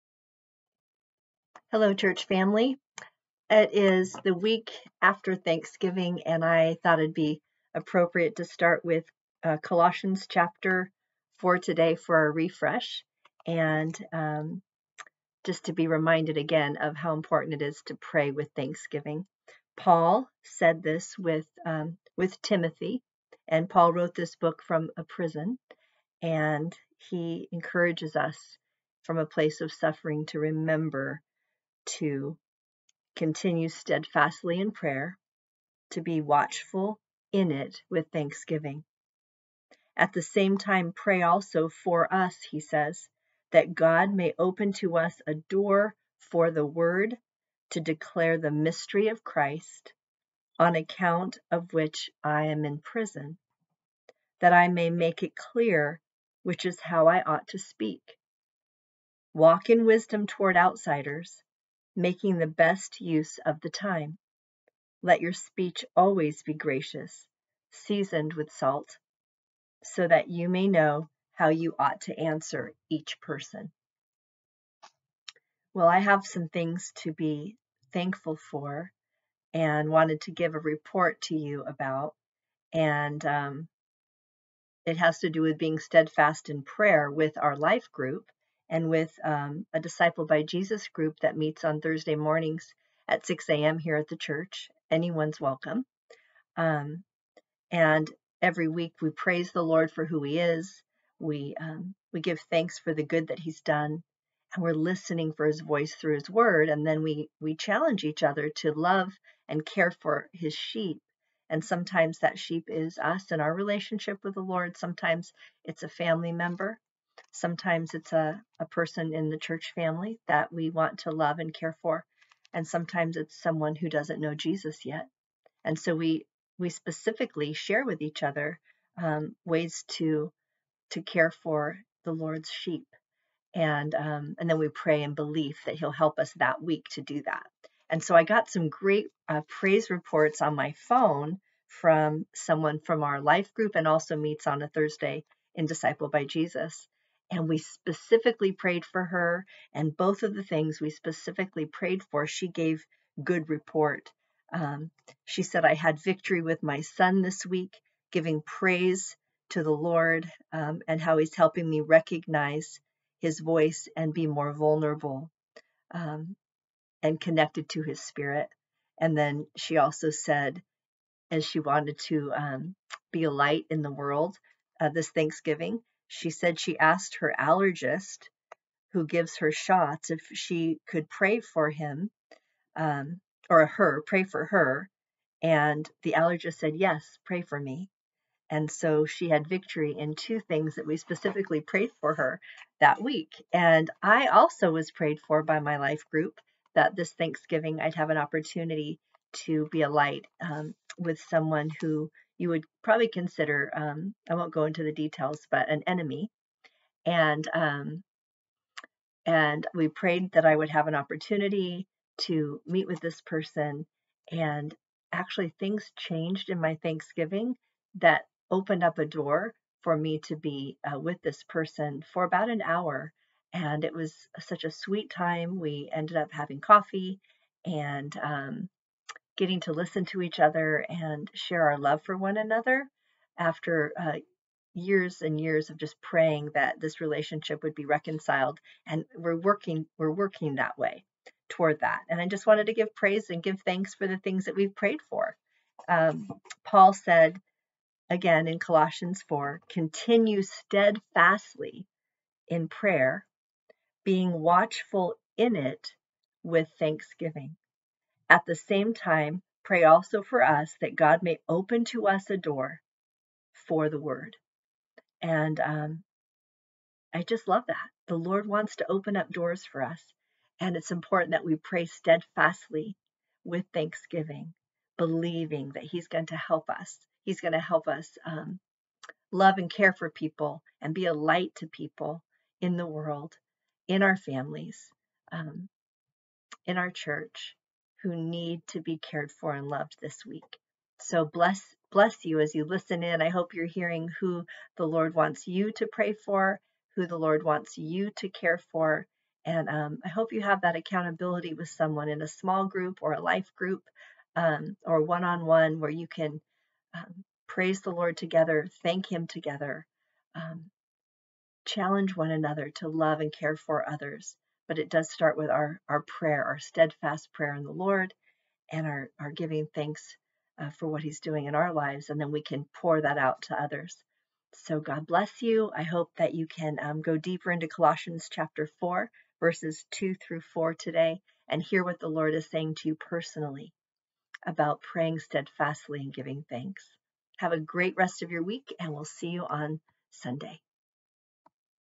OroNaz Refresh 11/29/23
The Refresh: OroNaz Devo  \\  Join us for a word of refreshment from God's Word with Pastor Dar as she reminds us to "continue in steadfast prayer with one another and to be watchful in it with thanksgiving." (Colossians 4:2-6)